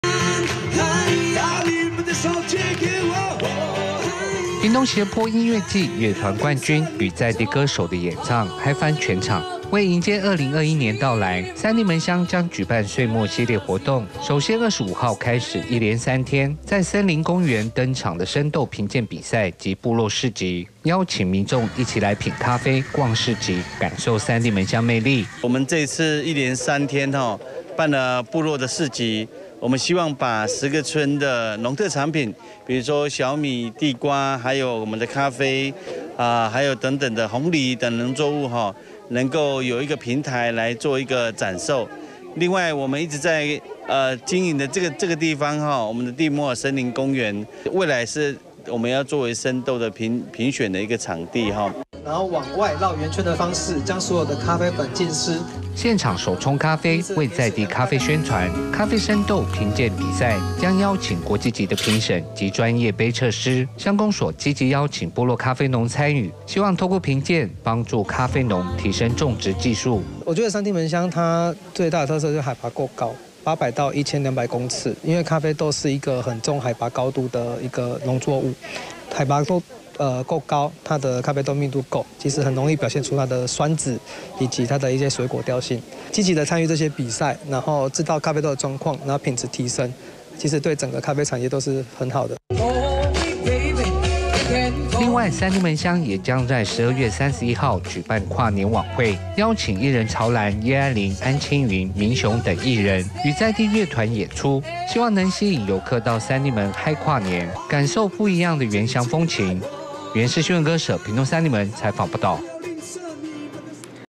三地門將辦歲末活動 熱鬧跨年迎接2021 2020-12-22 IPCF-TITV 原文會 原視新聞
屏東斜坡音樂季樂團冠軍與在地歌手的演唱嗨翻全場，為迎接2021年到來，三地門鄉將舉辦歲末系列活動，首先25號開始一連三天，在森林公園登場的生豆評鑑比賽及部落市集，邀請民眾一起來品咖啡、逛市集，感受三地門鄉魅力。

現場手沖咖啡，為在地咖啡宣傳，咖啡生豆評鑑比賽，將邀請國際級的評審與專業杯測師，鄉公所積極邀請部落咖啡農參與，希望透過評鑑，幫助咖啡農提升種植技術。

另外，三地門鄉也將在12月31日舉辦跨年晚會，邀請藝人曹蘭、葉璦菱、安欽澐、民雄等藝人，與在地樂團演出，希望能吸引遊客到三地門嗨跨年，感受不一樣的原鄉風情。